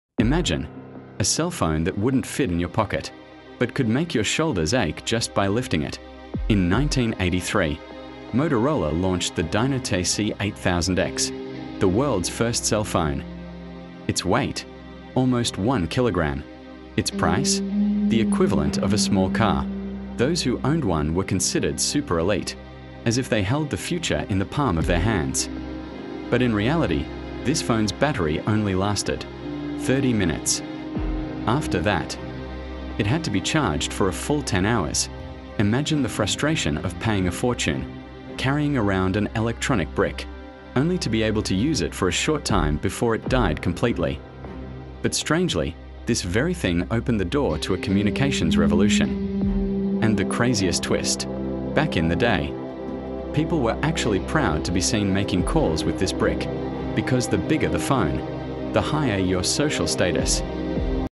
The First Cell Phone Weighed Like a Brick
Did you know that the technology we use every day has a shocking history? From the first telephone that was mocked, to computers that once cost as much as a car, and even WiFi whose inventor was almost forgotten.

In this video, we’ll uncover the hidden stories behind the evolution of technology — filled with drama, controversies, and mind-blowing facts you’ve probably never heard before.

🔥 Get ready, because some of these revelations will completely surprise you!